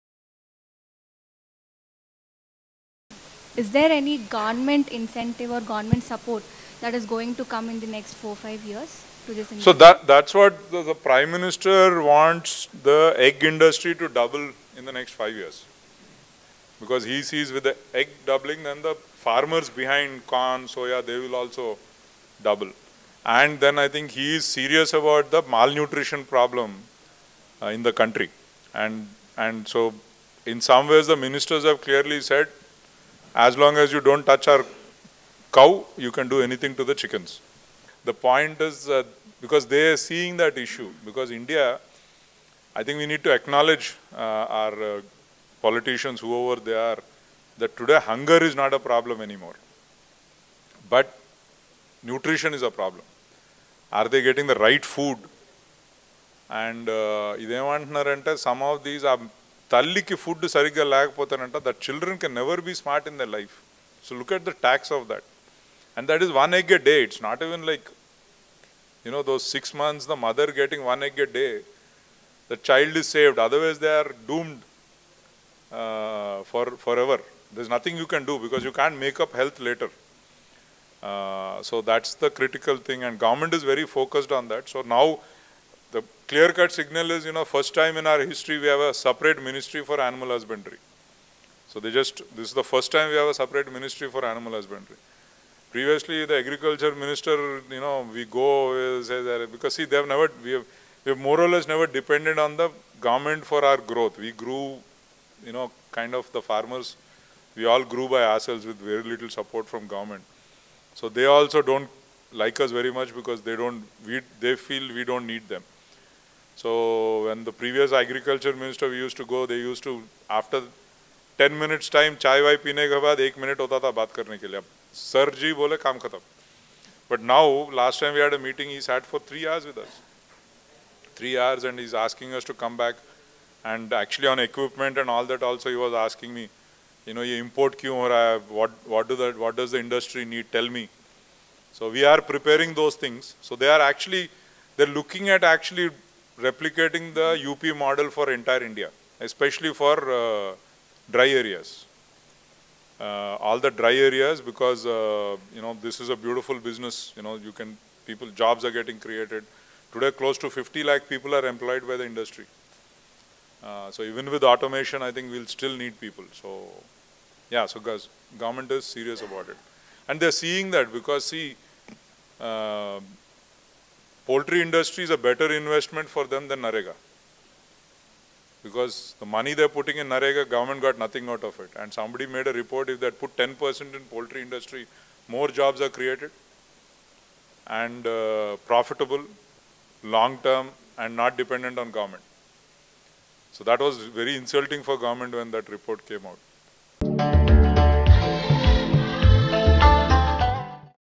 Any Government support to Egg Industry in India | TATA Wiron Poultry Parivaar Meet 2019
Suresh Rayudu Chitturi,  MD Srinivasa Farms Ltd, Vice-Chairman - International Egg Commission @ TATA Wiron Poultry Parivaar Meet 2019 at Ramoji Film City Hyderabad